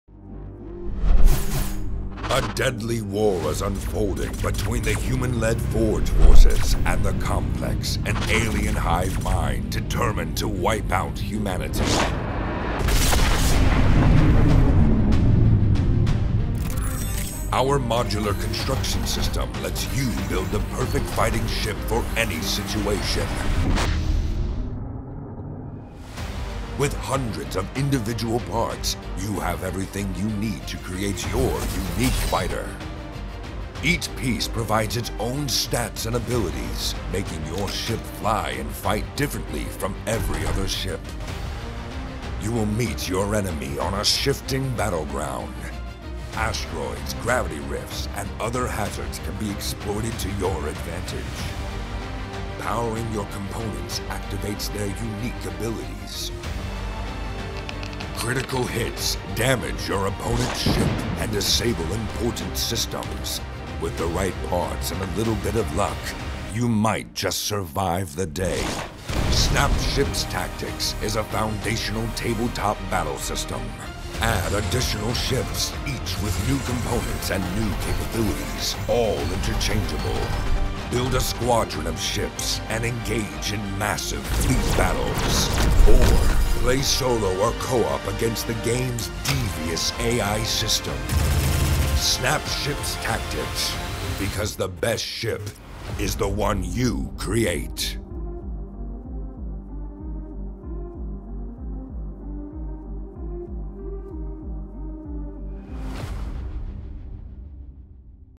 Snap Ships Tactics Kickstarter Video
Snap Ships arrive on the tabletop in Snap Ships Tactics, a miniatures battle game you play with modular ships you build from the ground up! Versus, Co-op, and Solo play are all supported!  Available now